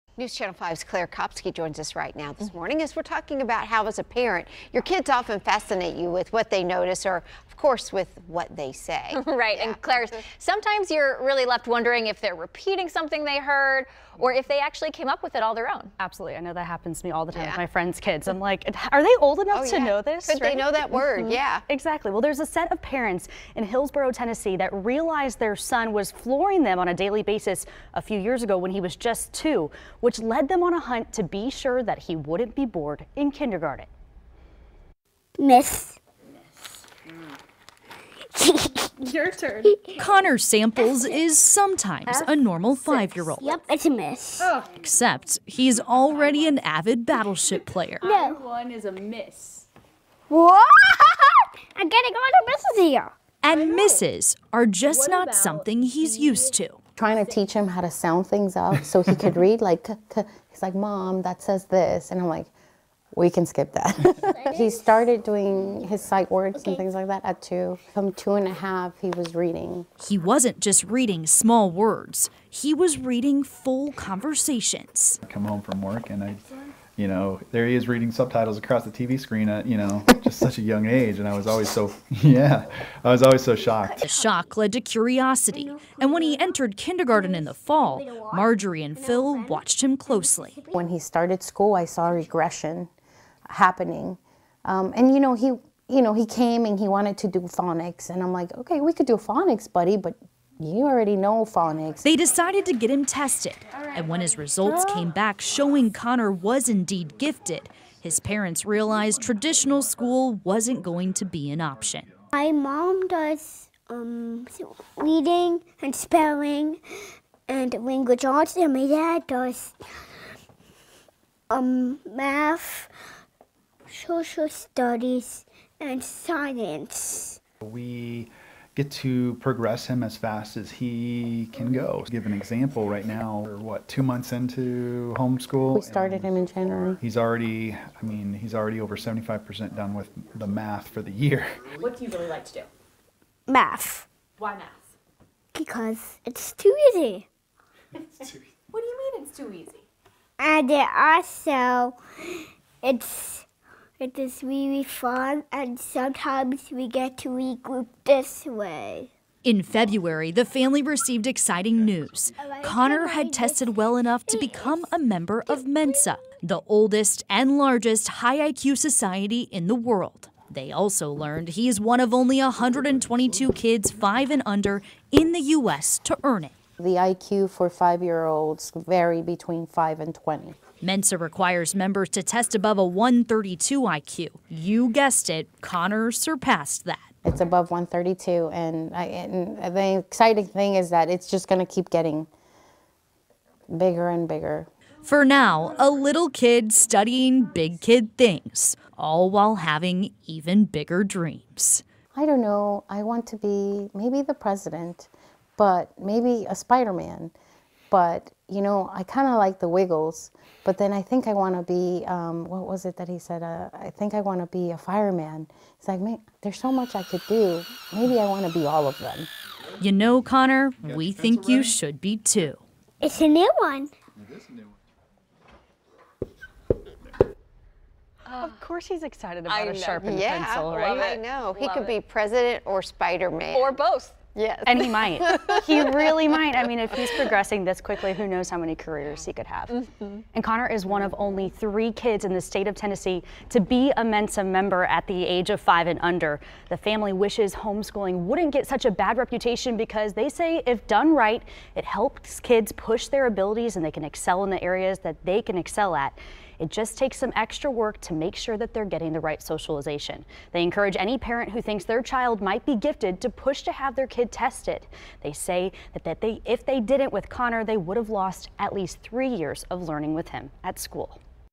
5-year-old in Tennessee earns Mensa membership after testing for high IQ
Five-year-old Connor Samples from Coffee County, Tennessee earned his Mensa membership in February for testing above a 132 IQ.